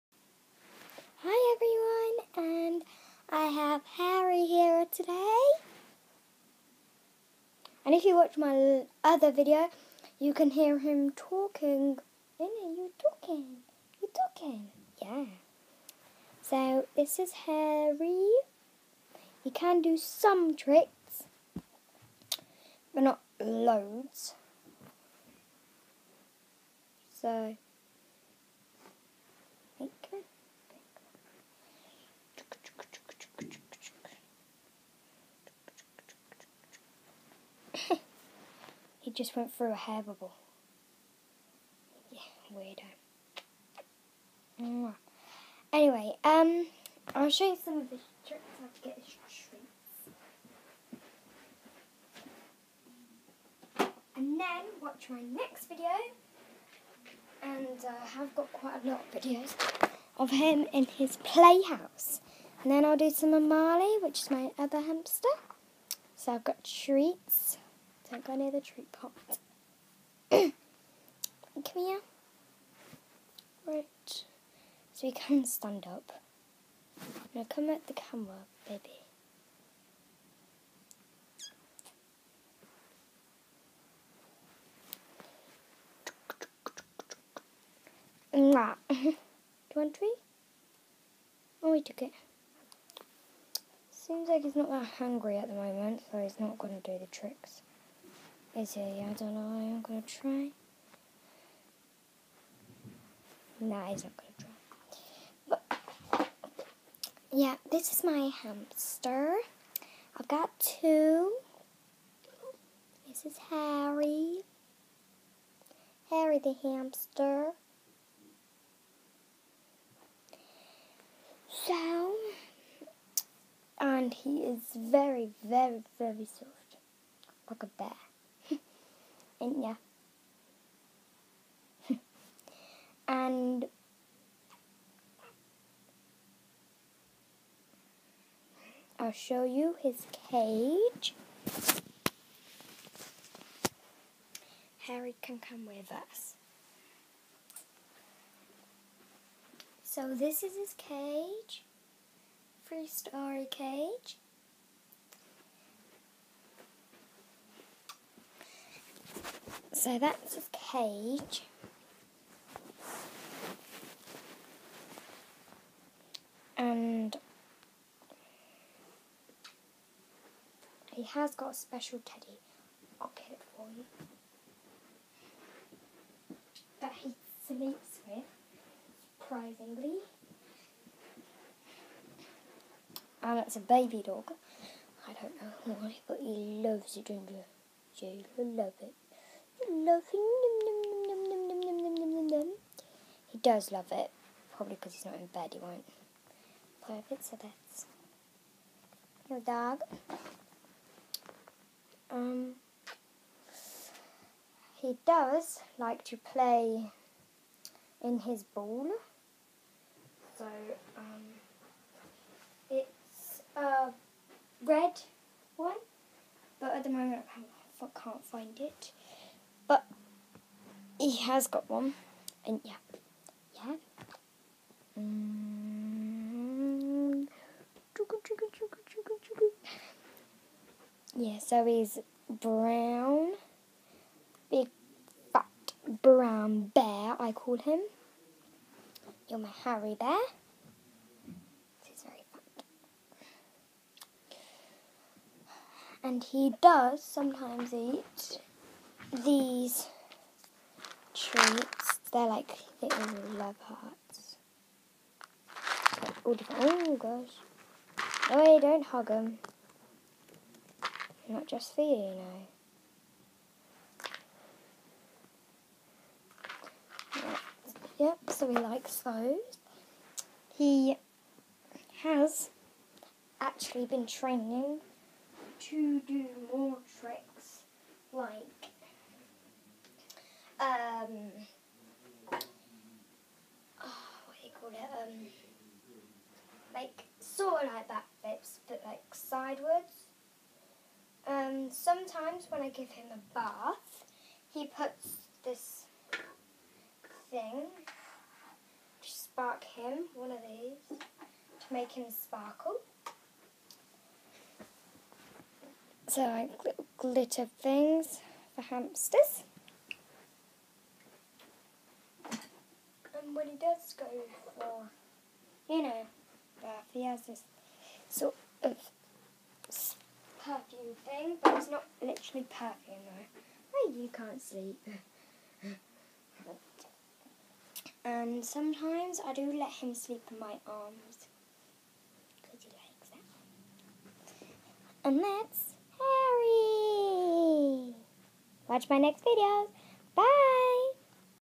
Meet Harry My Hamster
Harry is my lovable and fury friend. he is caring and i love him on this video he is 1 year and 2 months He has a friend named Marley Hamster and you can see him on the next video. Harry has a 3 story cage and he loves it lots and he is very very spoilt. I call him Harry Bear because he is big brown and he looks like a tiny tiny bear. Thanks and bye . Bye